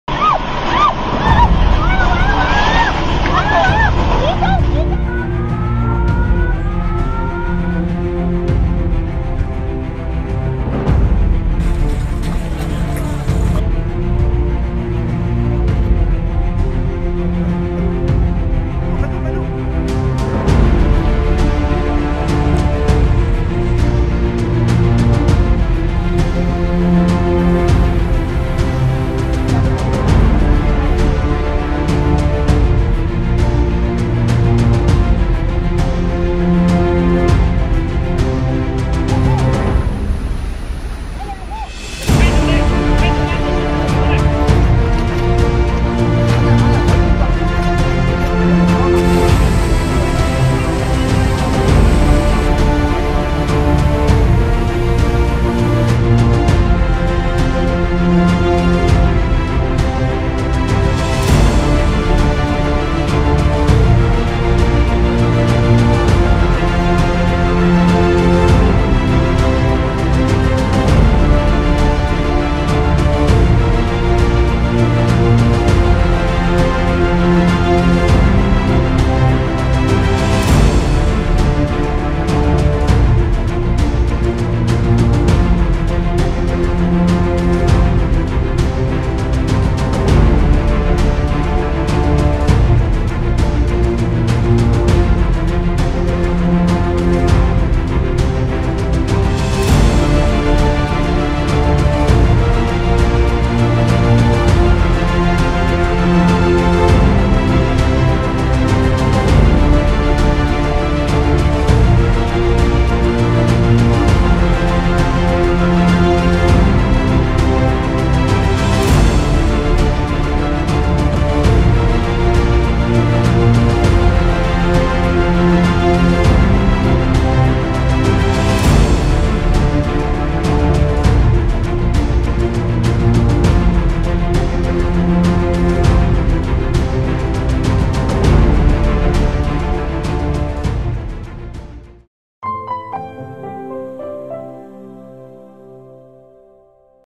直击四川地震救援这些震撼瞬间
9月5日12时52分，四川甘孜州泸定县发生6.8级地震。多支救援力量赶赴现场展开救援。
道路长、山路阻，踩着滚落的石块，灾害面前，时间就是生命。武警、消防等救援人员勇毅前行。